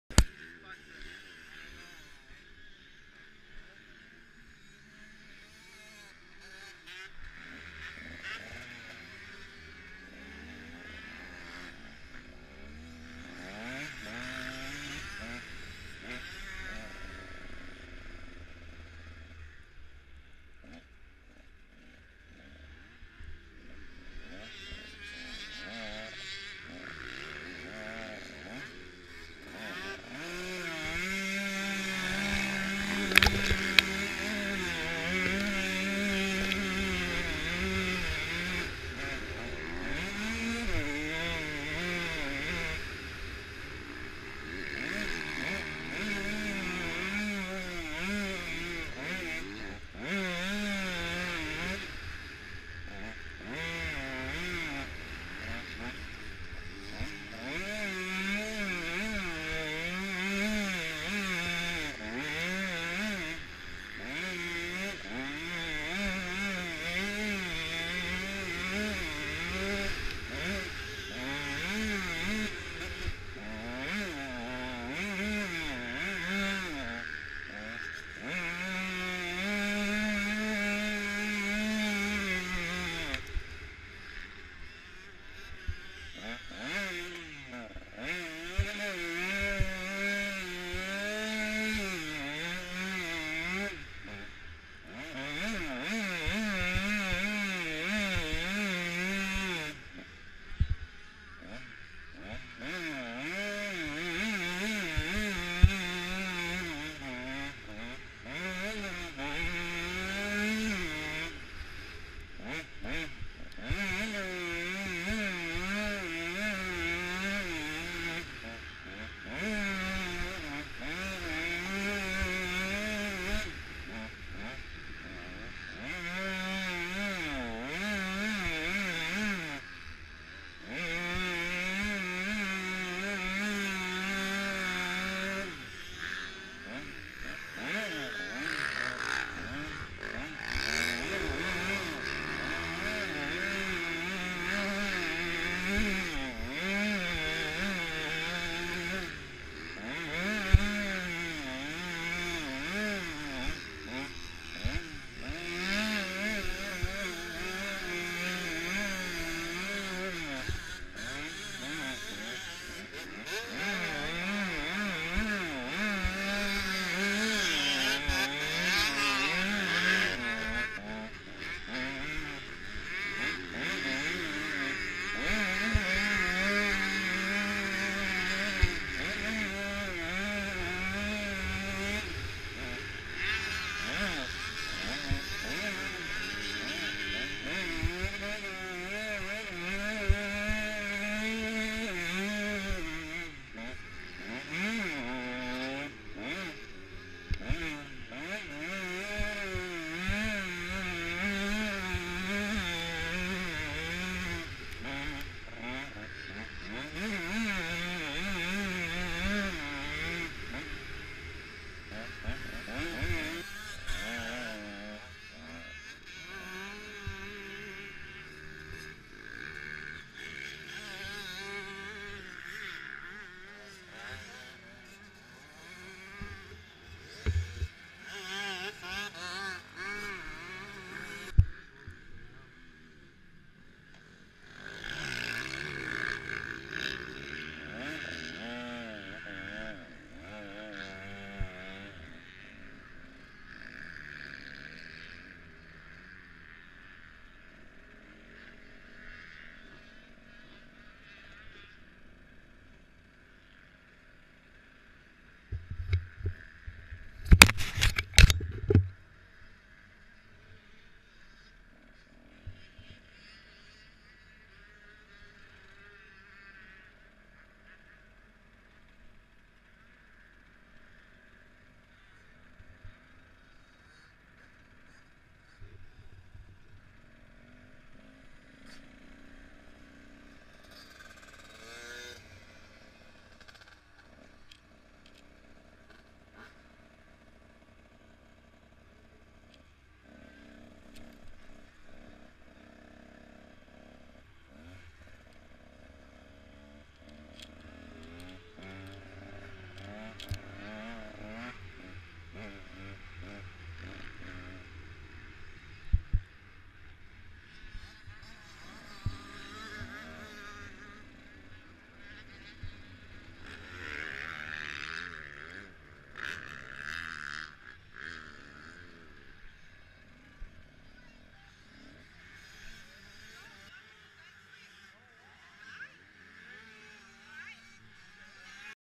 Our Lew at Lommel MX easter 2015
propper rough about 5 mins after being graded, was so wet day before looked like the tide had come in